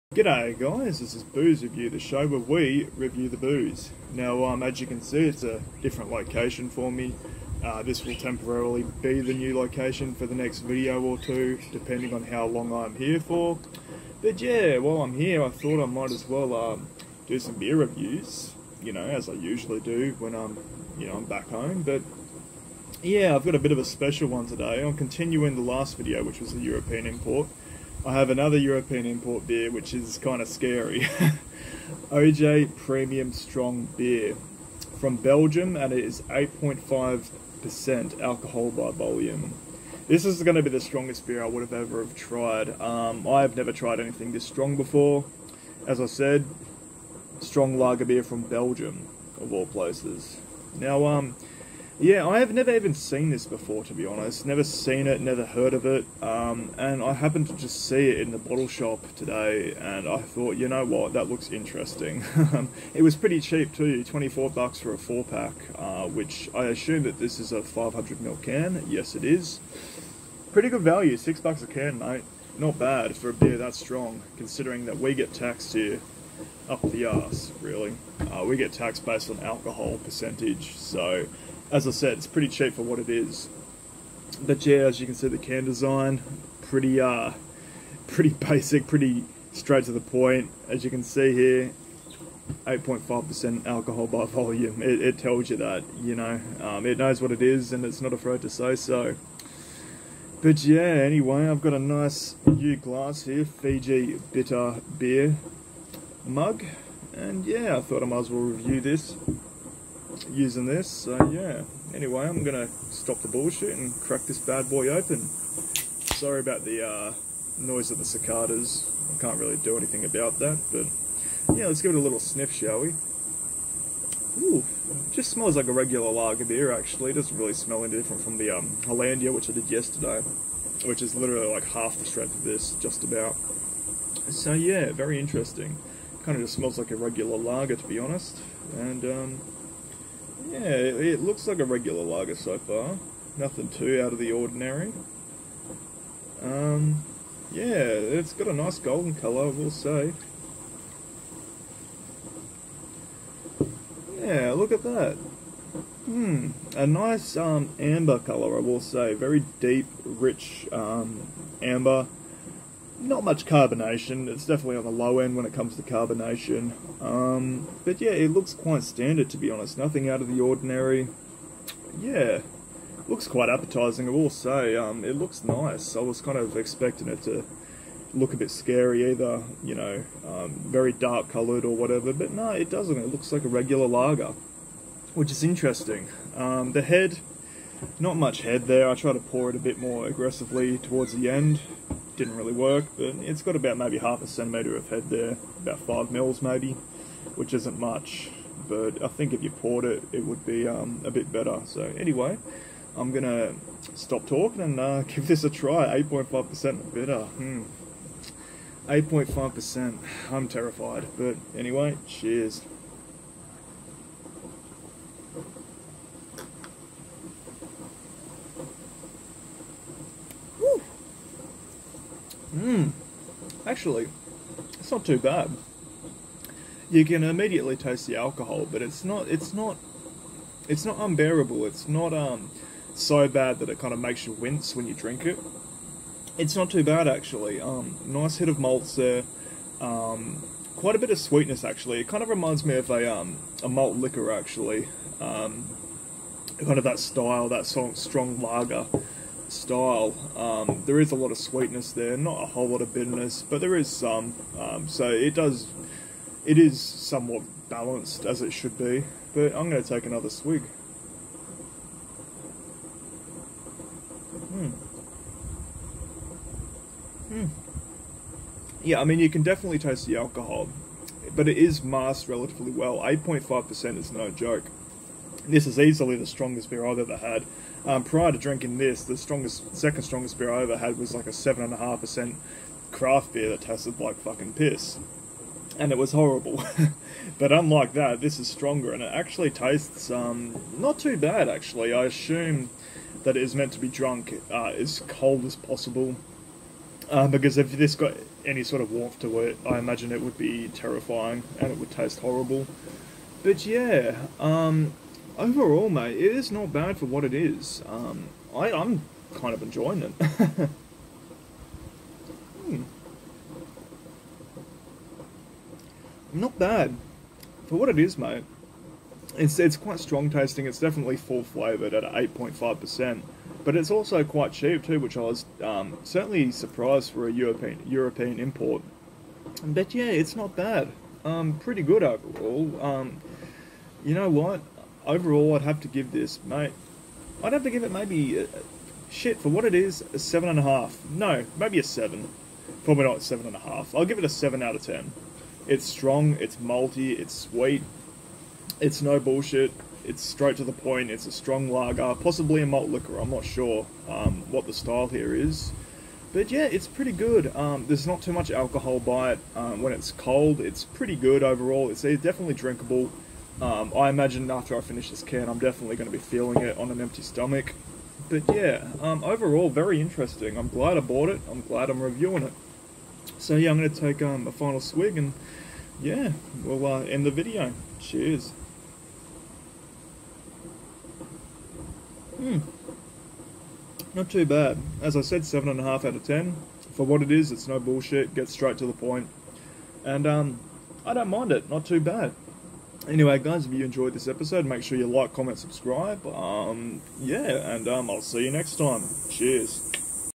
O.J. Premium Strong Beer - Beer Review
Strong, sweet and malty. For what it is, it ain't too bad overall. 8.5% ABV in a 500ML can.

I'll have to rank this a 7/10 or a "B-".